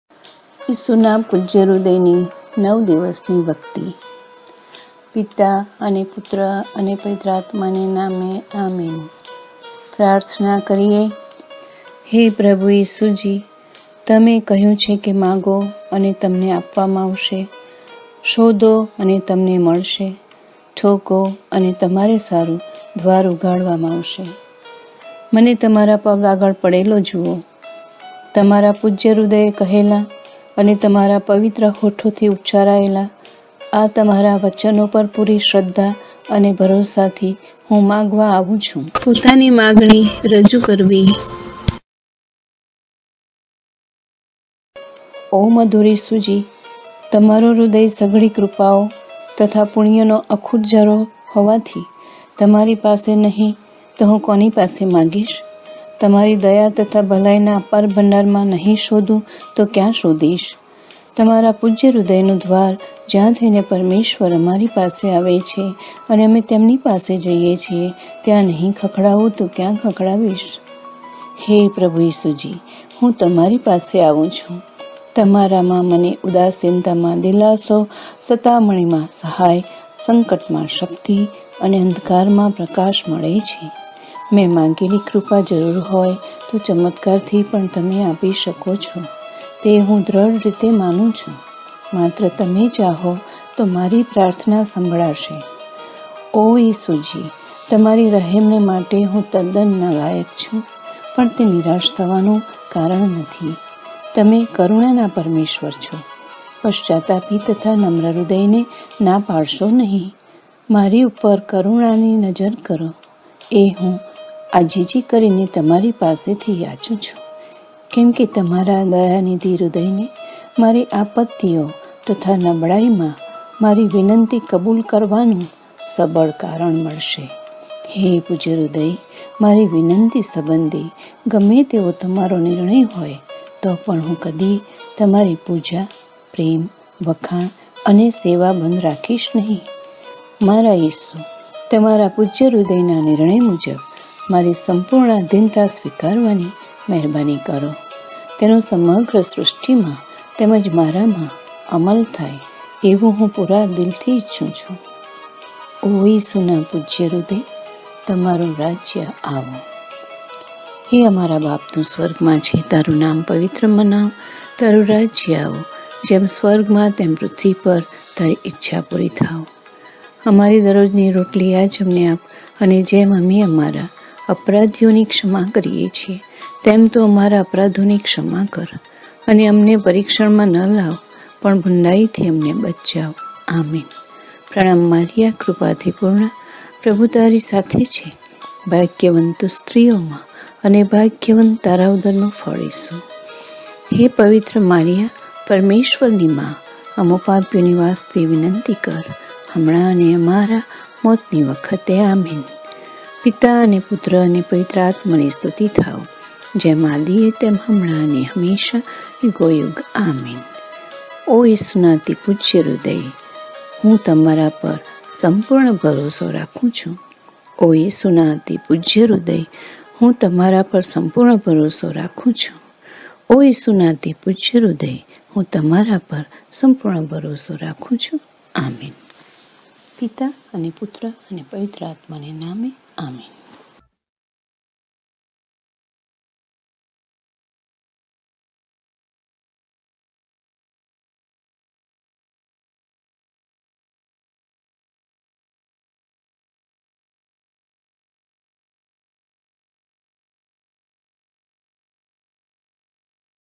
#"માંગો અને તમને મળશે "પ્રભુના આ વચન પર ભરોસો રાખી # શ્રદ્ધાપૂર્વક ઈસુના પૂજ્ય હૃદયની ભક્તિ કરવી.#
જૂન મહિનો પ્રભુ ઇસુના અતિ પવિત્ર હૃદય પ્રત્યેની ભક્તિનો મહિનો મનાય છે. એનું મુખ્ય કારણ એ છે કે પવિત્ર હૃદયનો તહેવાર આ મહિનામાં ઉજવવામાં આવે છે.# આ મહિનામાં રોજ પૂજ્ય હૃદયની ખાસ ભક્તિ થાય એવો પવિત્ર રિવાજ ધર્મસભાએ ઠરાવેલો છે#. તેથી બની શકે તો દરરોજ પ્રભુ ઈસુના પૂજય હૃદયની ભક્તિ કરવી.#